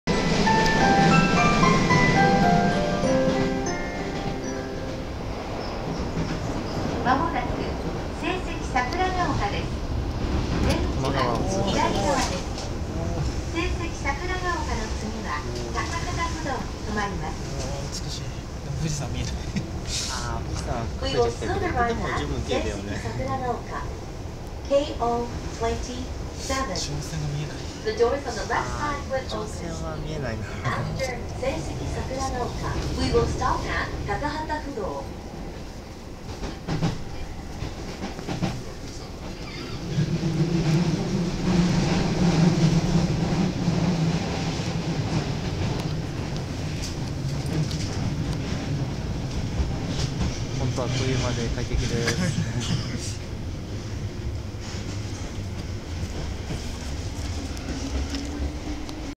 京王ライナー乗車4
まもなく聖蹟桜ヶ丘。到着前には多摩川を通過です。